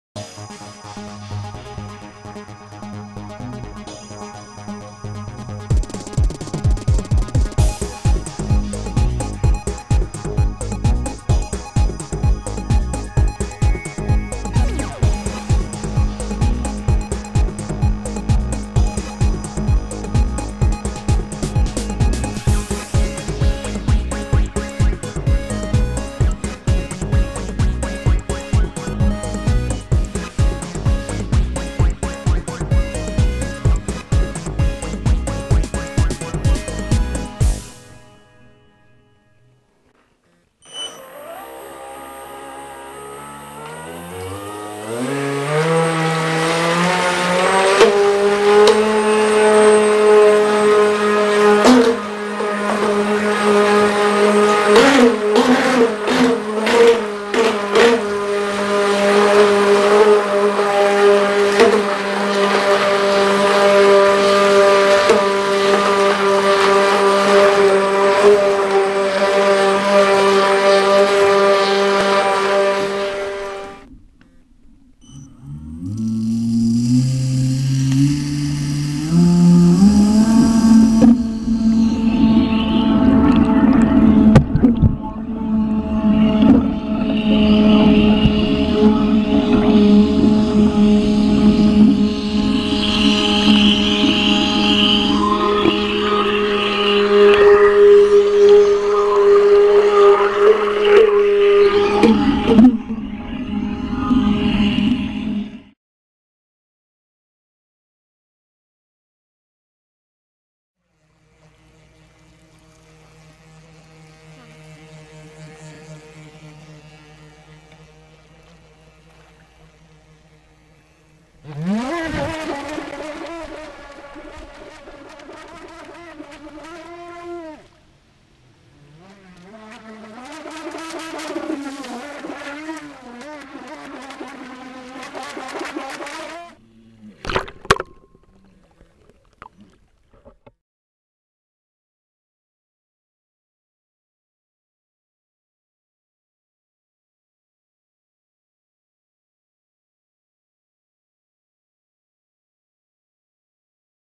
Testing the amps on my Reef Racer 2.wmv
Go here for more information and discussion Checking the amps to make sure that it will not be too high for the motor and ESC